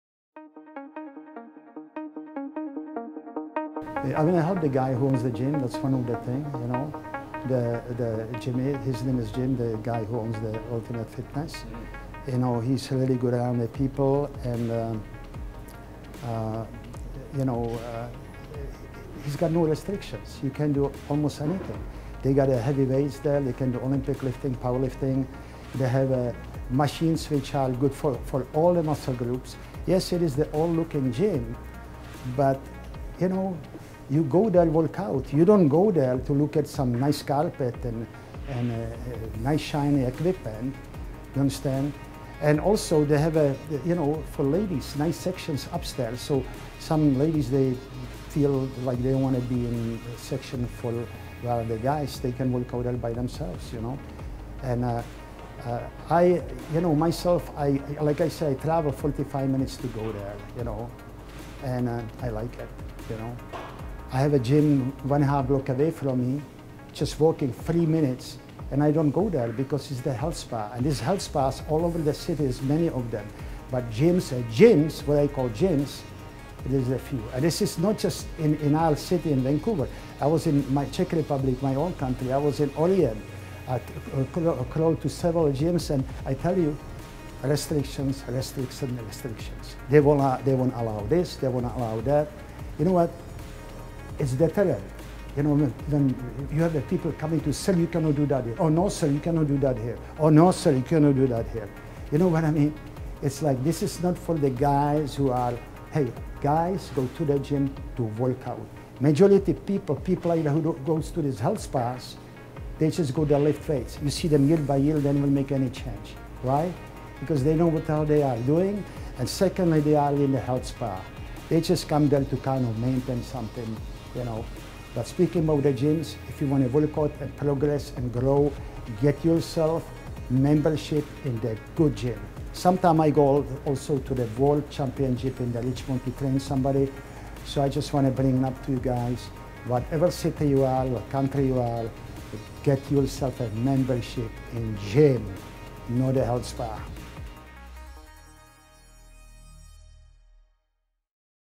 Difference between Gyms and Health Spas by Sila Athletic
When I came to Vancouver there were only three gyms in the city. Old gyms by todays standards Olympic weight lifting, Powerlifting, Bodybuilding. There were no restrictions on those days. Since then many gyms start opening up, at first they were pretty good, but gradually gyms were changing to more like health spa types. Facilities with many restrictions. Accommodating only certain members. Today real GYM are few. ULTIMATE FITNESS is one of few real Gyms around. Management is very good with members and there are no restrictions.